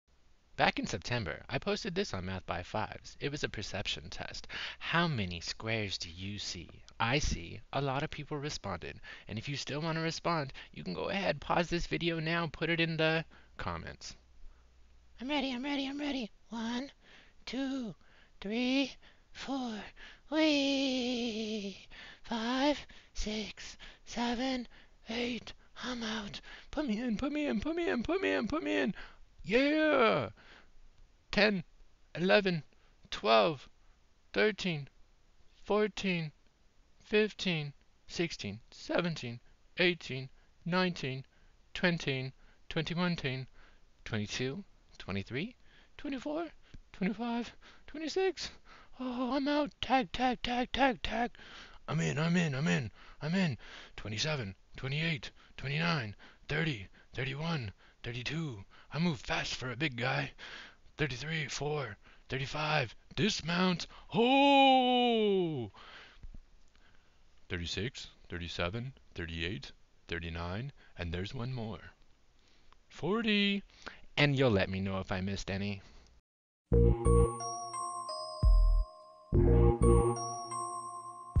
Counting Boxes with Flowers
How many squares do you see? There are no rules, this is a perception test, you may see it differently than I do.